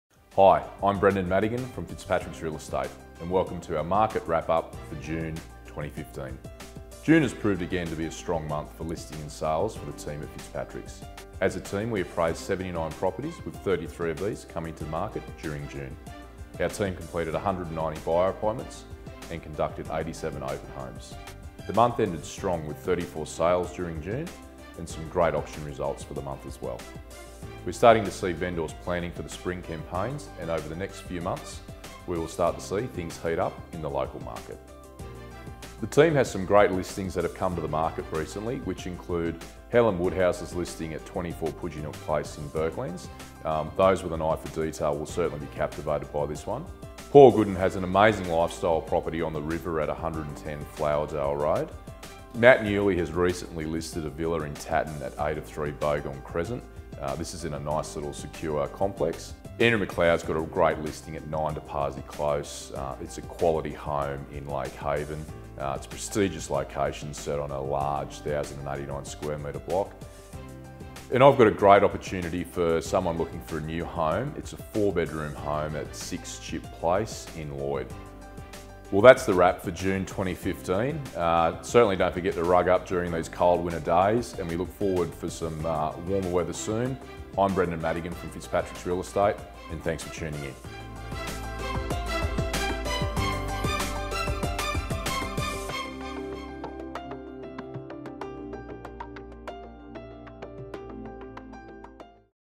Wagga Residential Sales Market Wrap - July 2015
Fitzpatricks Director and sales consultant Brendan Madigan shares an inside glance at Wagga Wagga’s current real estate market.